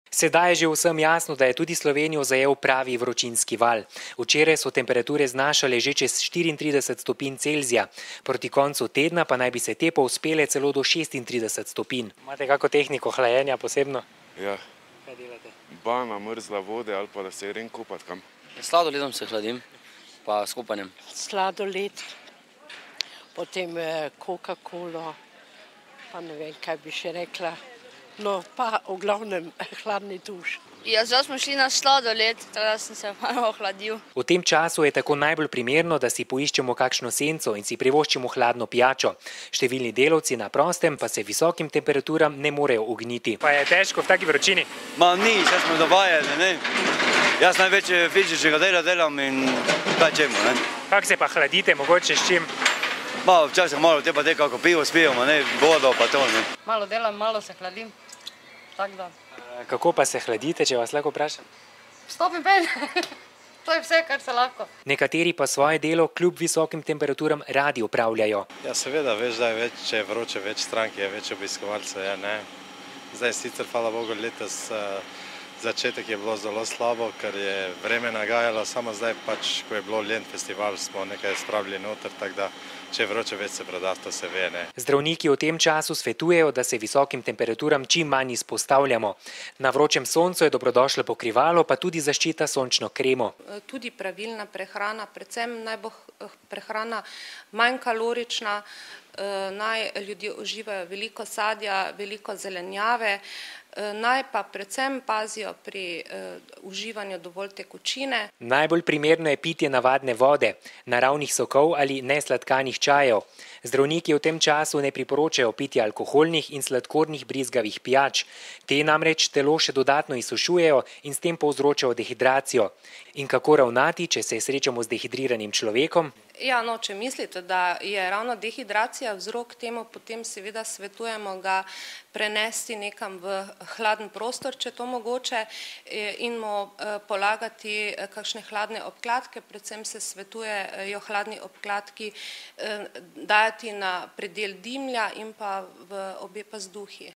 Ko poči termometer
Tudi v Mariboru smo zajadrali v vročinski val, temperature so se namreč povzpele krepko čez 30 stopinj Celzija. Življenje v mestu tako v tem času postaja prava mora, Mariborčani pa se najpogosteje hladijo s sladoledom, hladno pijačo ali skokom v bazen.

"Danes in jutri bo precej jasno, popoldne bodo predvsem v hribovitem svetu nastale vročinske nevihte. Najnižje jutranje temperature bodo od 14 do 20, najvišje dnevne od 29 do 34 stopinj Celzija," so zapisali na ARSO.

Podobno vreme pričakujejo tudi v naslednjih dneh, ko naj bi se se živo srebro na termometrih povzpelo celo do 36 stopinj. Vročina naj bi tako trajala vse do sobote, ko naj bi se pojavile manjše osvežitve, vendar ne resne ohladitve, so sporočili z ARSO.

V nadaljevanju preverite, kako se hladijo Mariborčani in kaj v tem času priporoča zdravniška stroka.